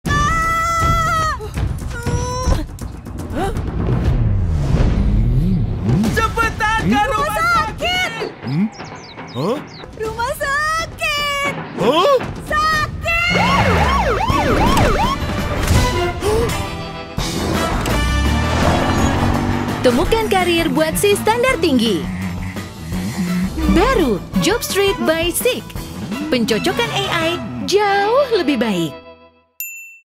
Kejar Mimpi Pakai Teknologi AI
Jago ngitung angka tapi kerjanya di bidang kreatif. Suka bikin konten tapi kerjanya malah ngolah data angka.

Dengan fitur AI canggih dari Jobstreet, nggak ada lagi yang namanya salah pilih kerjaan, karena teknologi AI Jobstreet akan bantu kasih rekomendasi dari total 40.000+ loker yang cocok sesuai dengan profil Jobstreet kamu.

Download aplikasi Jobstreet di Play Store atau App Store sekarang!